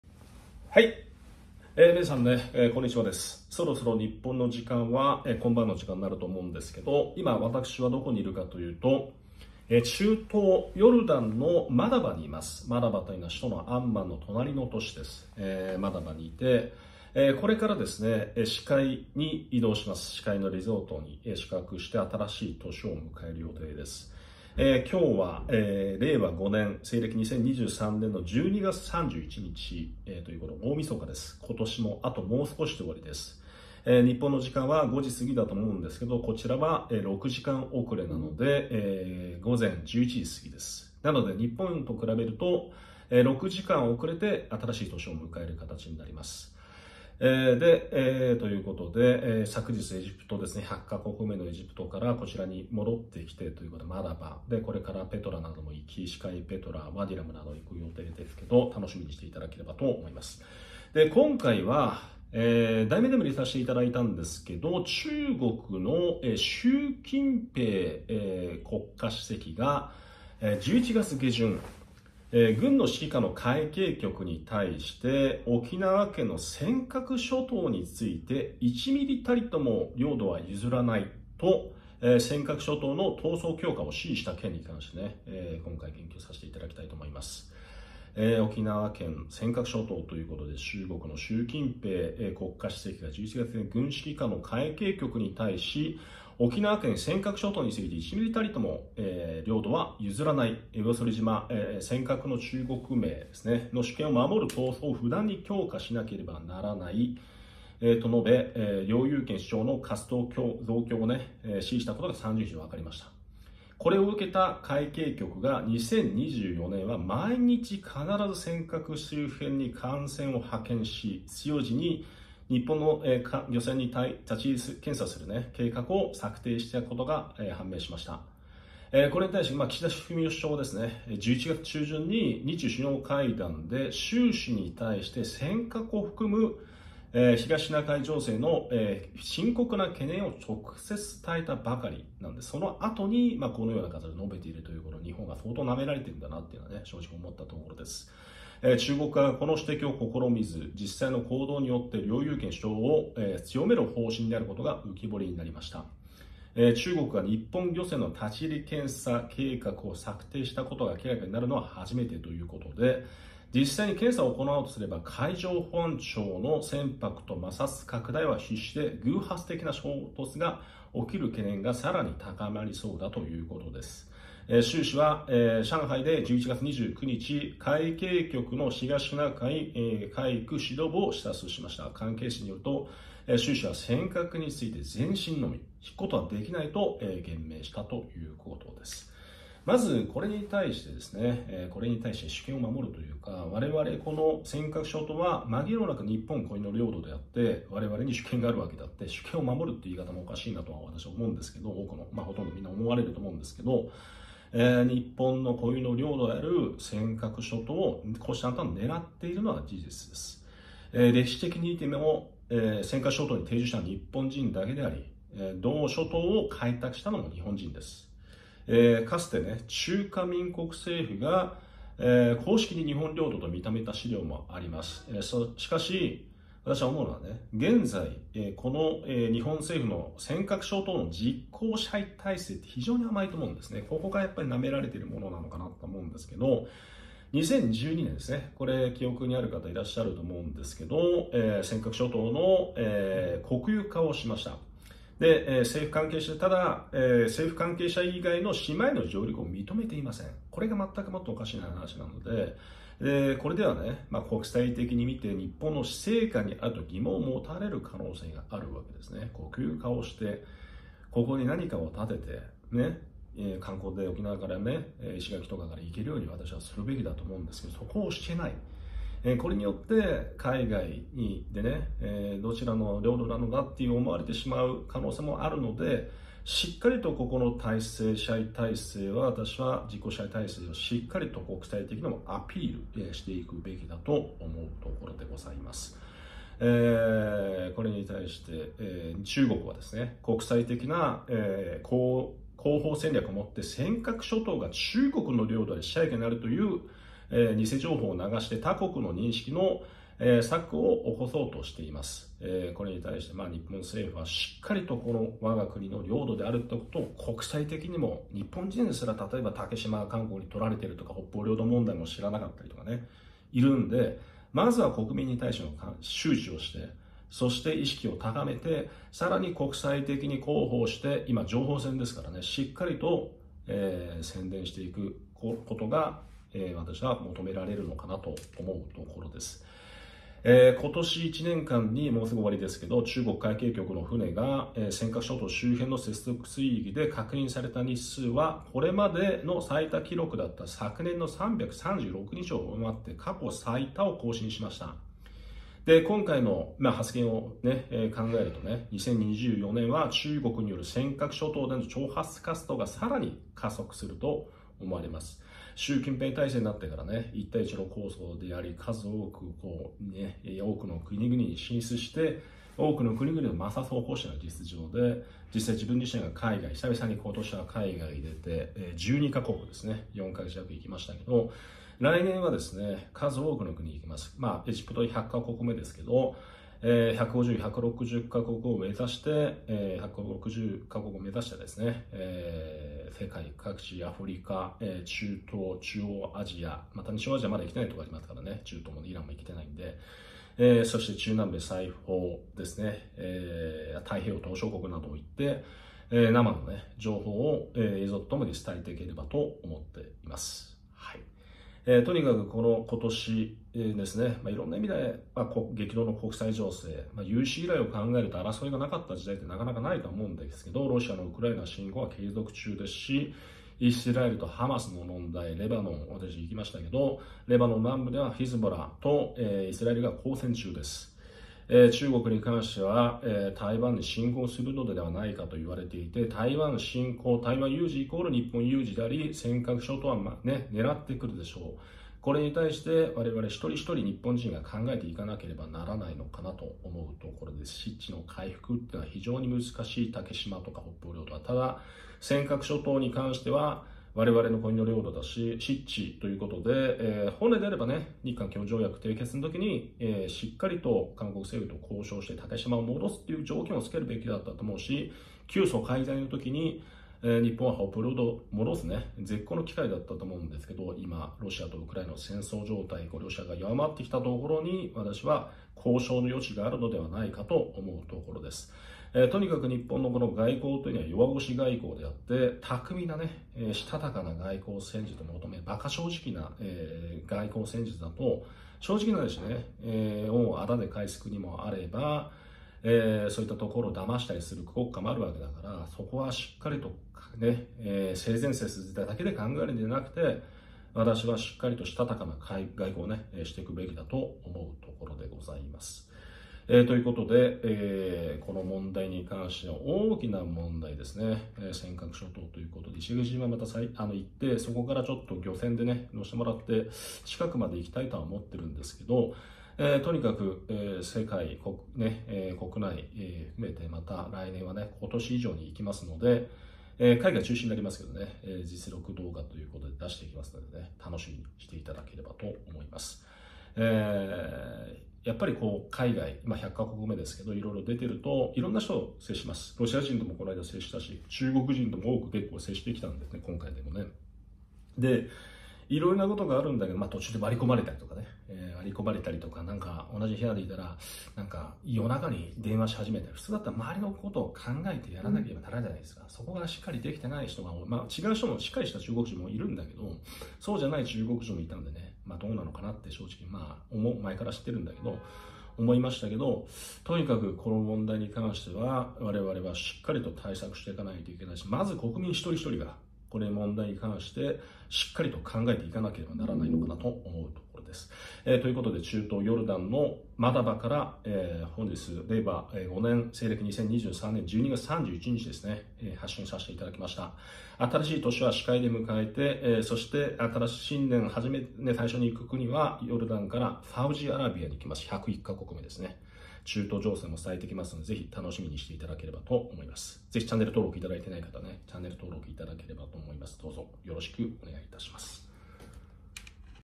中国の習近平主席が沖縄県‐尖閣諸島に対して「1ミリも領土は譲らない」尖閣諸島の闘争強化を指示した件！日本固有の領土である尖閣諸島-竹島、北方領土などの失地回復！台湾有事⁼日本有事！ヨルダンの死海にて
『12月30日-共同通信下記記事を引用して解説』
中国の習近平国家主席が11月下旬、軍指揮下の海警局に対し、沖縄県・尖閣諸島について「1ミリたりとも領土は譲らない。釣魚島（尖閣の中国名）の主権を守る闘争を不断に強化しなければならない」と述べ、領有権主張の活動増強を指示したことが30日、分かった。これを受け海警局が、2024年は毎日必ず尖閣周辺に艦船を派遣し、必要時には日本の漁船に立ち入り検査する計画を策定したことも判明した。

　岸田文雄首相が11月中旬の日中首脳会談で習氏に、尖閣を含む東シナ海情勢への「深刻な懸念」を直接伝えたばかり。中国側がこの指摘を顧みず、実際の行動によって領有権主張を強める方針であることが浮き彫りになった。

　中国が日本漁船の立ち入り検査計画を策定したことが明らかになるのは初めて。実際に検査を行おうとすれば、海上保安庁の船舶との摩擦拡大は必至で、偶発的な衝突が起きる懸念がさらに高まりそうだ。

　習氏は上海で11月29日、海警局の東シナ海海区指揮部を視察した。関係筋によると習氏は尖閣について「前進のみ。引くことはできない」と言明した。